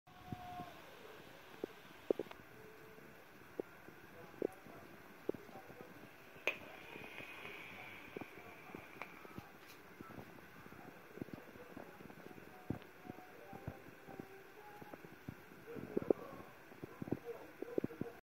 Preparation Pierogis Zubrowska.
Une zubrow aux herbes et une preparation classique de pierogis.
La pate farine oeuf..on rajoute un peu d'eau.
La farce..oignons..fromage blanc bien essoré et patates ecrasées.
On peu les faire a tout..myrtilles..etc..
On peut les cuire et les congeler..
Car c'est pas difficile mais long et ca prend de la place.